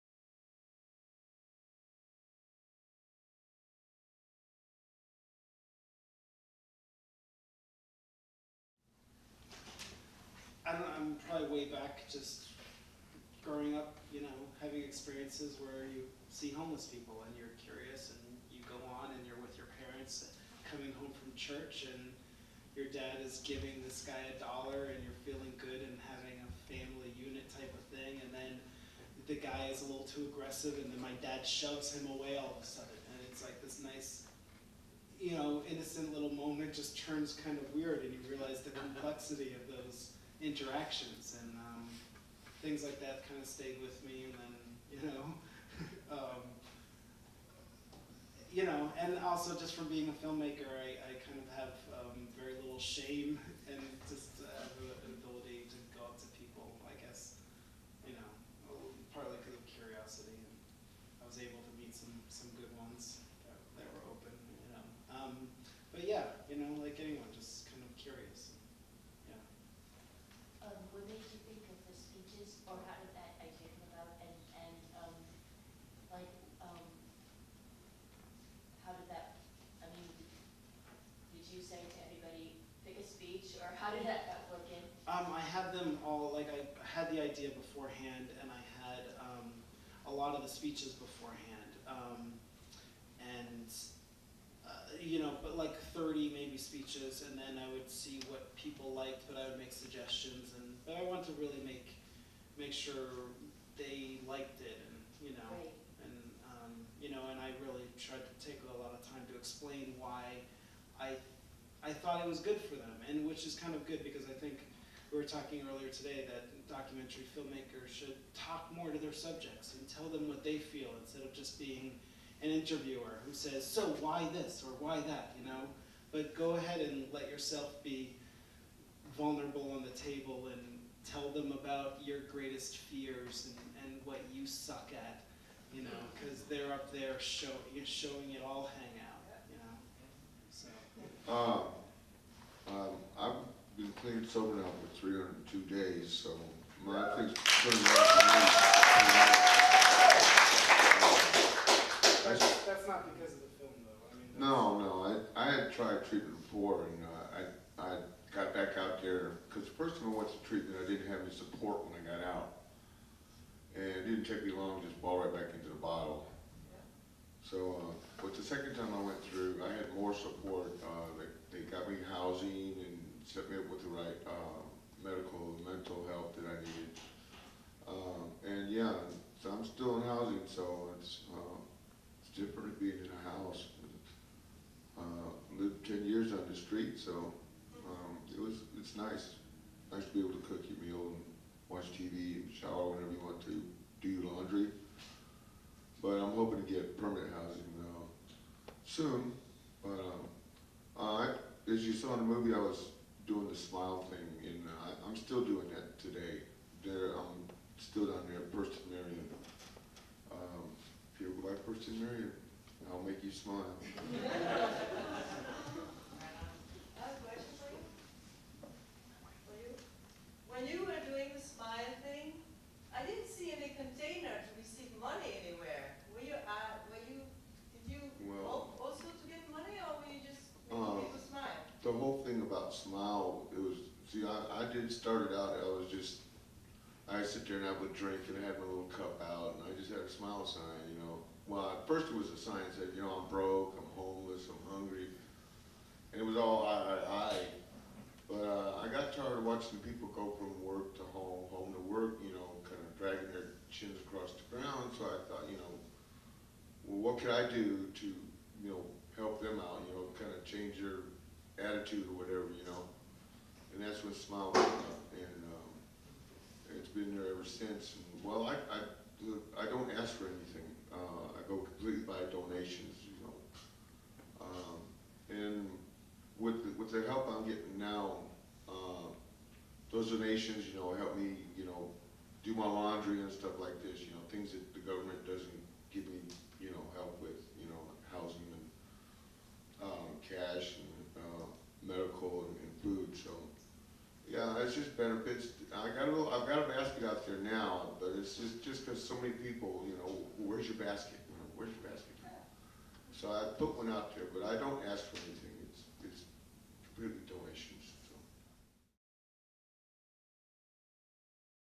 Linas Phillips Q&A
Director Linas Phillips and subject Tomey talk about GREAT SPEECHES FROM A DYING WORLD